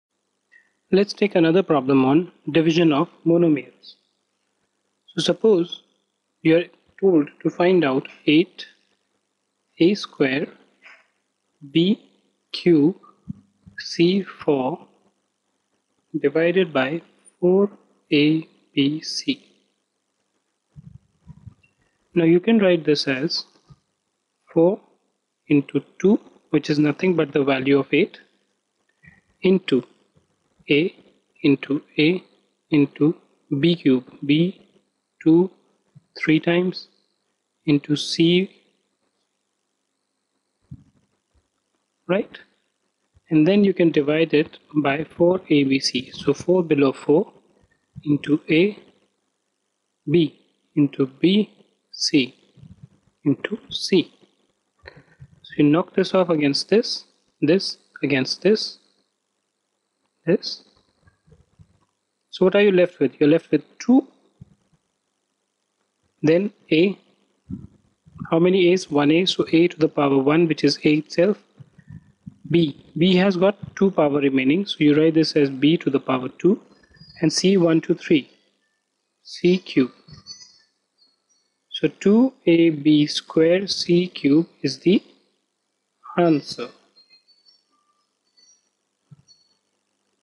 Division of Monomial by Monomial - Problem 1 - Algebric Expression - Maths
Learn About - Division of Monomial by Monomial Problem 1 - Algebric Expression - Maths - Class 8/VIII - ISCE|CBSE